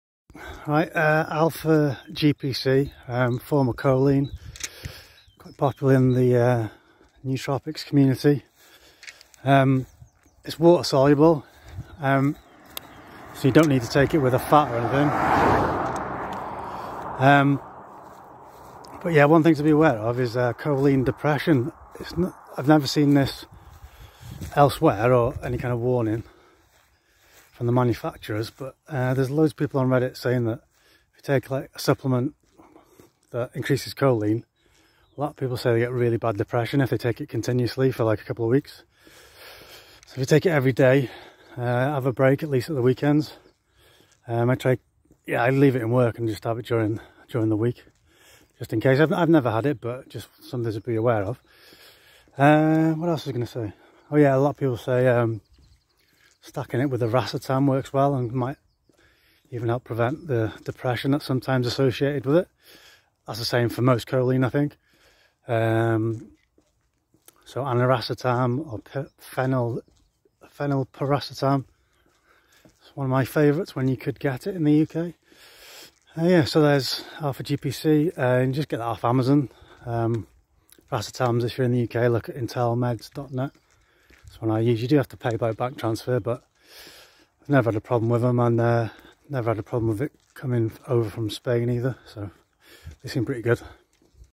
Alpha GPC [Andrew Huberman Recommended] Side Effect Warning! ⚠️
daily alpha gpc can cause choline depression.